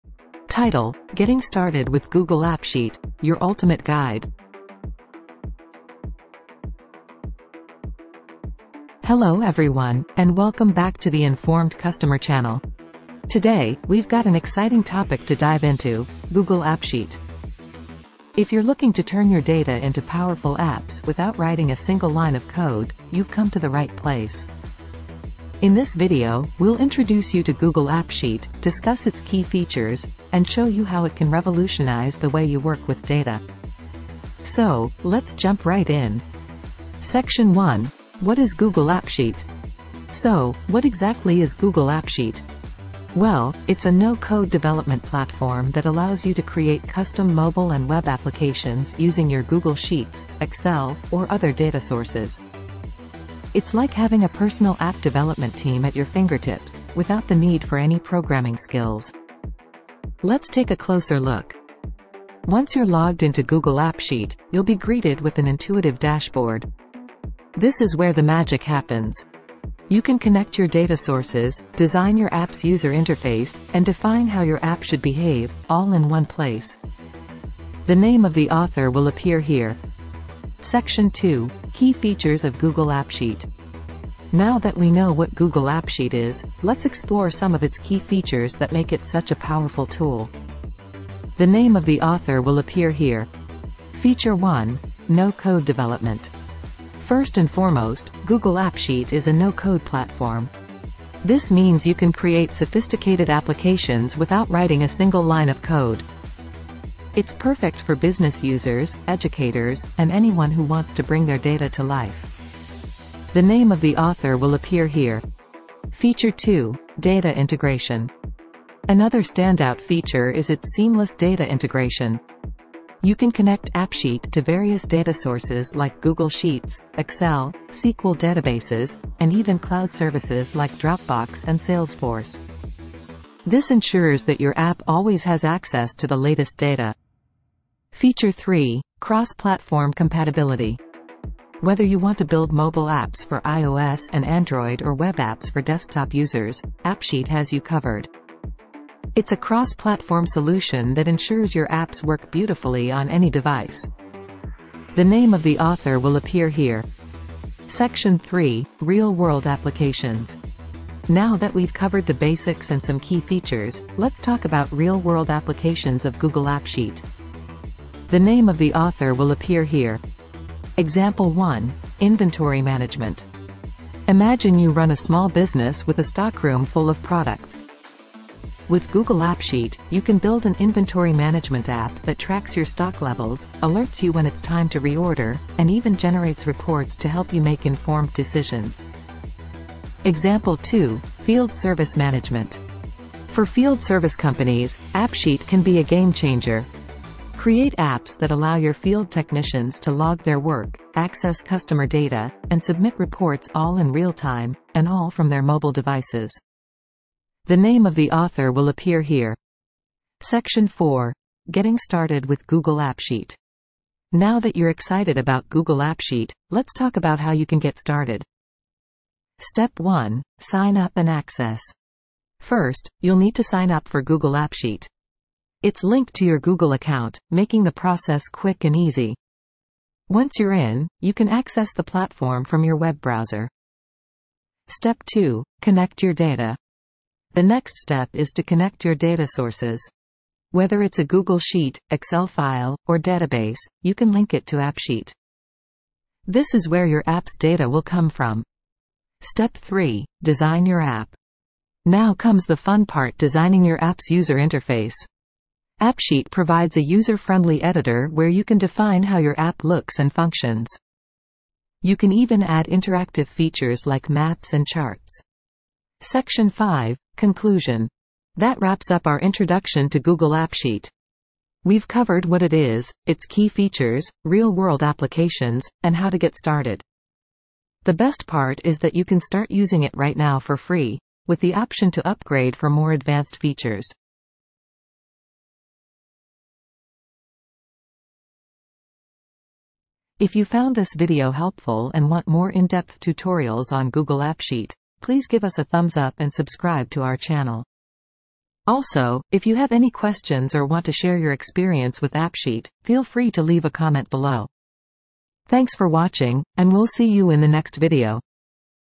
"Getting Started with Google AppSheet: Your Ultimate Guide"
Welcome to our comprehensive beginner's guide to getting started with Google AppSheet! Whether you're a business owner or an app developer, this ultimate guide is tailored to help you make the most out of this powerful platform. With AppSheet, you can create customized business apps without any coding knowledge, empowering you to streamline operations and scale your enterprise.